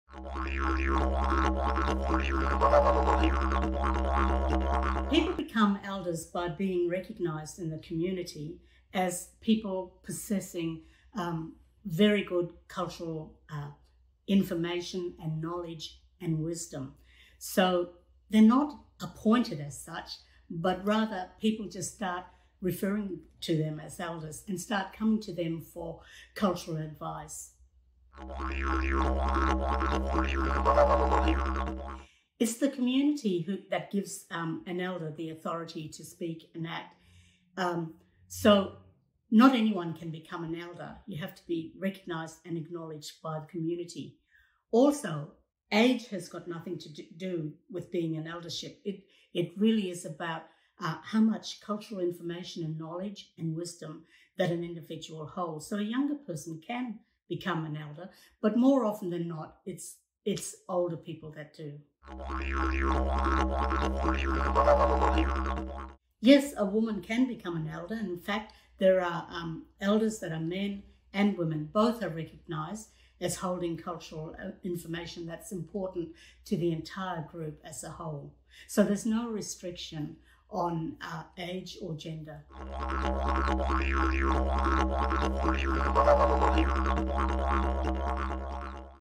Aboriginal Eldership - part1
👣 How does somebody become an Aboriginal Elder?

👣 Who gives Elders authority to speak and act?

👣 Can a woman be an Elder?

📺 Aunty Munya answers all your questions about Eldership in this week's video.

🤔 Do you have questions about Aboriginal and Torres Strait Islander people and culture?

👉🏾 Head on over to our website and submit your question to Dear Abby.